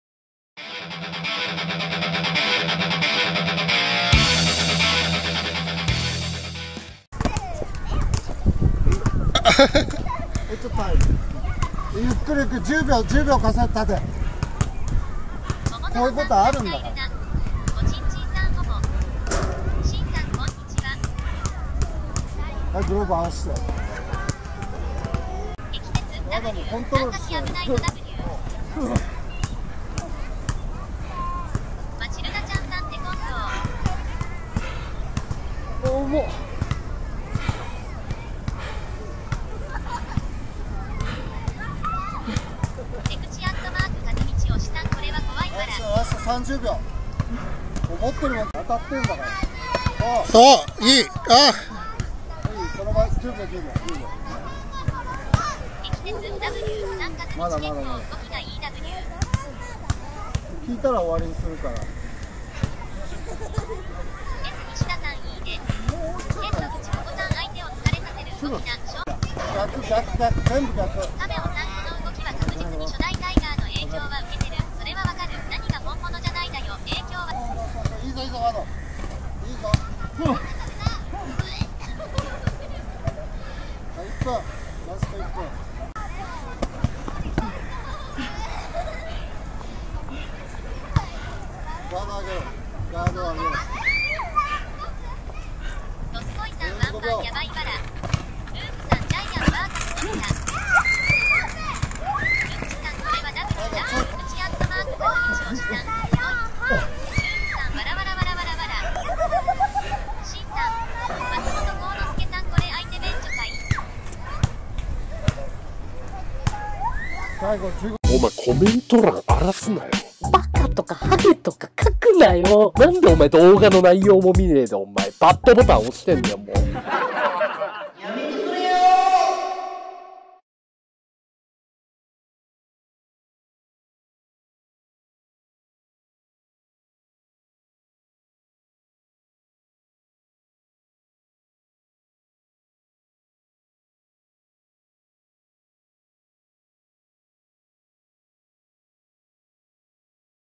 【爆笑KO】キックボクシングのスパーリングでダウン
キックボクシングの試合を控えた選手が生まれて初めてのスパーリングを行う。
しかし３ラウンドで７回もKO（ノックダウン）されてしまう。
対戦相手は喧嘩無敗（ストリートファイト）のタイガーマスク

【絶妙コンビネーション！】井岡一翔 職人芸KO ベスト10 Best10 Knockouts of Kazuto Ioka